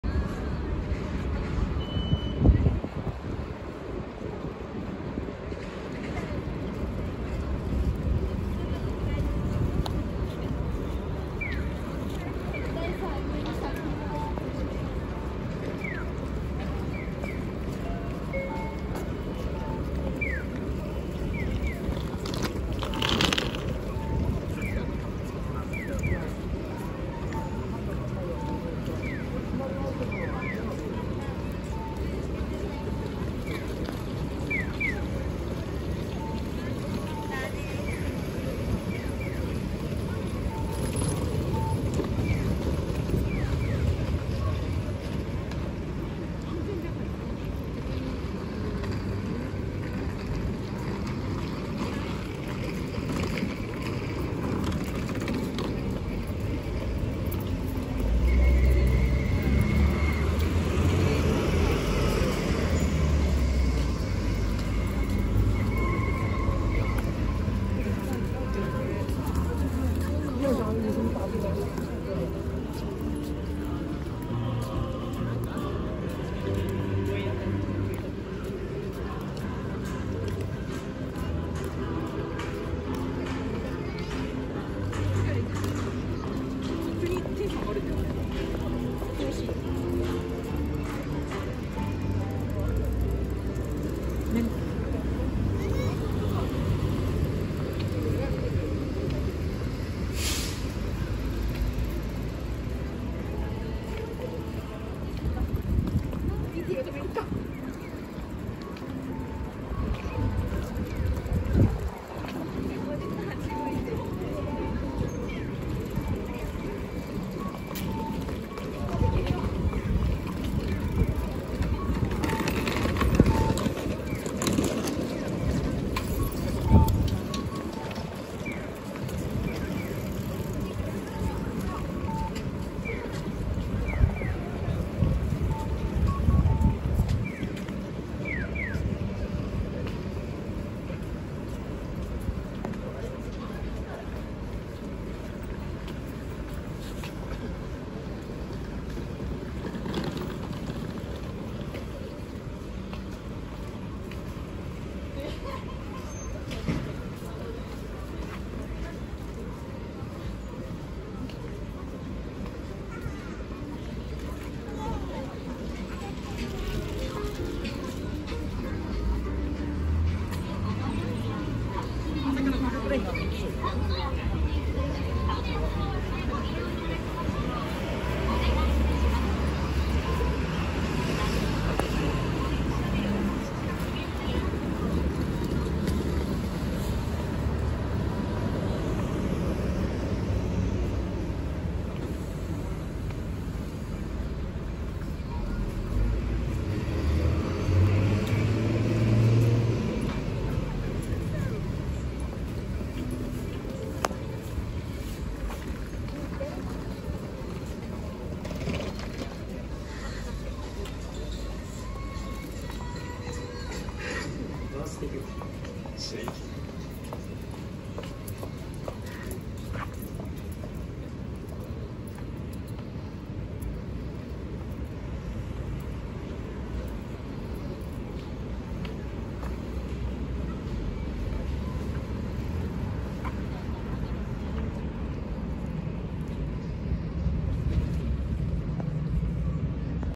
【4K池袋】池袋日本Ikebukuro 4K Walk | Walking Tour Around Ikebukuro, Tokyo_Japan 2022
This video is Taken by @japancitywalk  around Ikebukuro Station, Walking Tour.